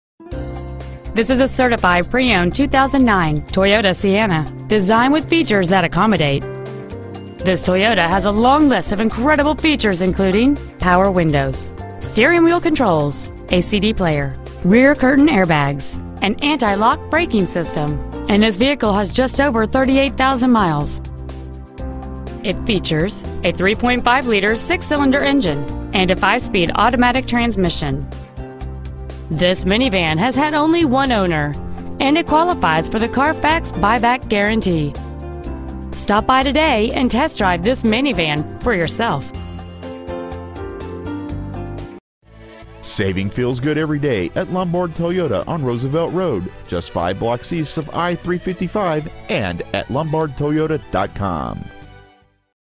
2009 Toyota Sienna Lombard IL
Year: 2009
 Make: Toyota
 Model: Sienna
 Engine: Gas V6 3.5L/211
 Trans.: Automatic
 Exterior: Slate Metallic
 Miles: 38,526
 Interior: Stone

 725 W. Roosevelt Road

 2009 Toyota Sienna Lombard IL